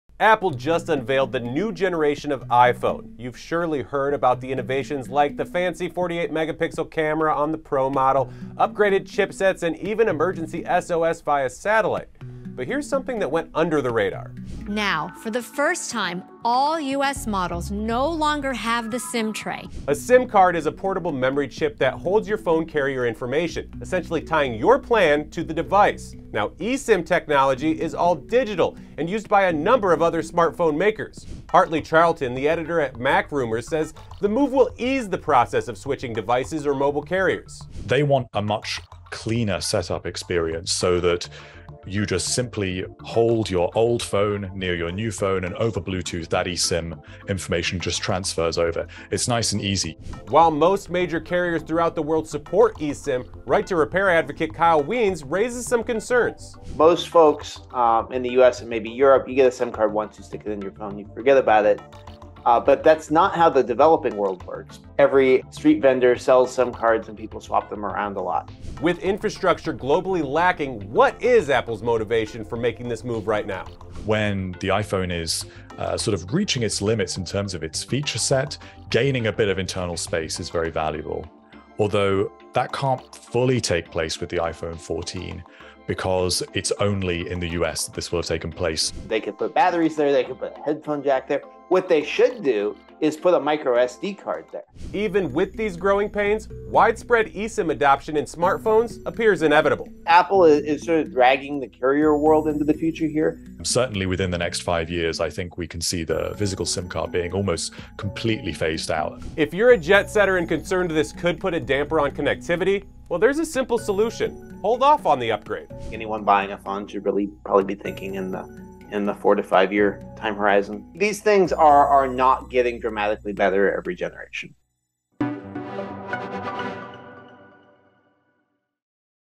Dragging carriers into the future, Apple ditches SIM card tray in iPhone 14
Apple’s iPhone 14 and iPhone 14 Pro offer a number of new features, including upgraded cameras, faster processors and emergency SOS via satellite. But dropping the physical SIM card tray in favor of a digital eSIM in U.S. models could have the biggest impact across the mobile phone landscape.

The Subscriber Identity Module card, or SIM card, is a portable memory chip that holds mobile carrier information. The card essentially ties the device to the provider and assigns a phone number for calls and messages. eSIM functions the same digitally, generally over Bluetooth, without the need for a physical chip.